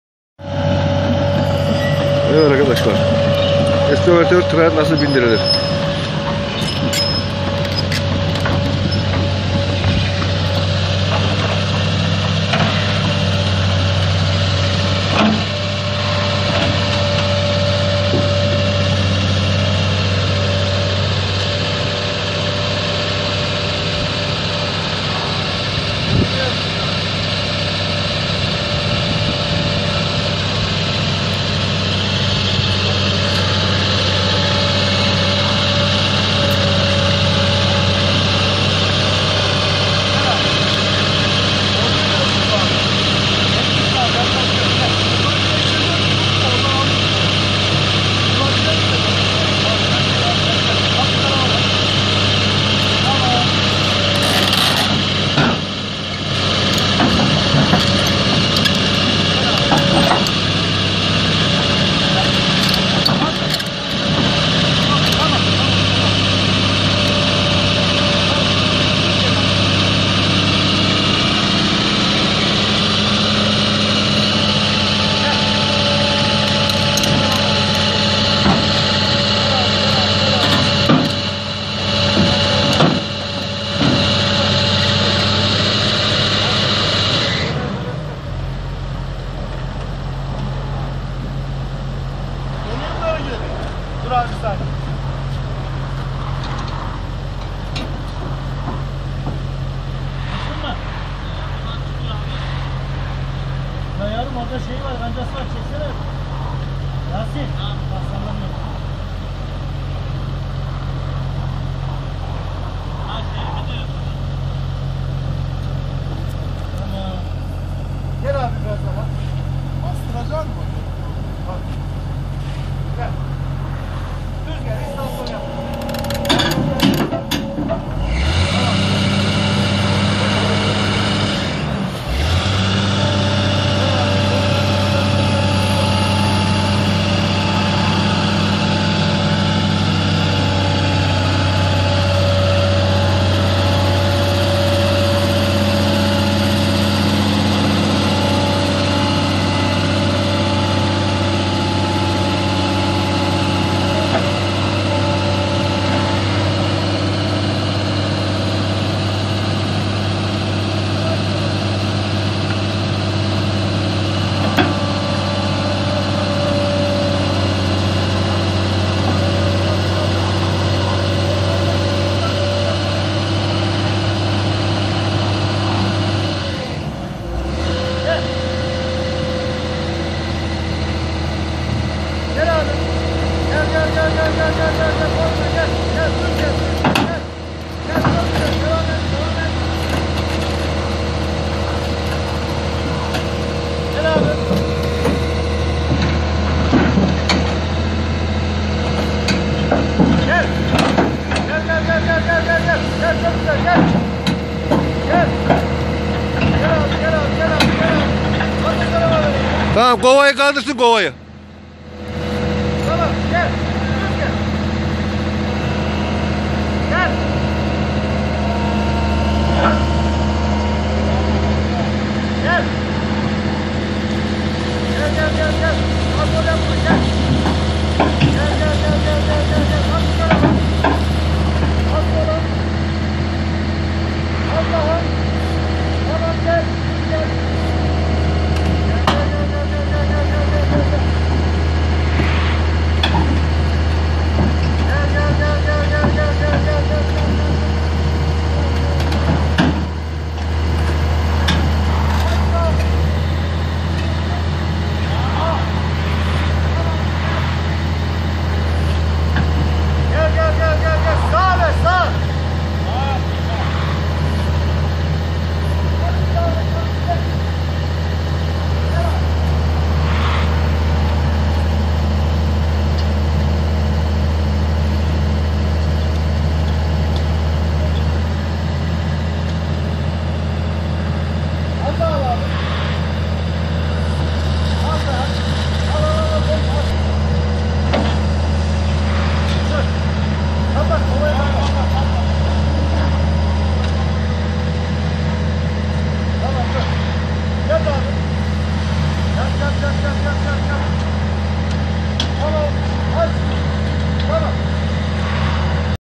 EXKAVATÖR TIRA NASIL BİNDİRİLİR - HOW TO STORE THE EXCAVATOR TRAILER
JCB 200 - HOW TO STORE THE EXCAVATOR TRAILER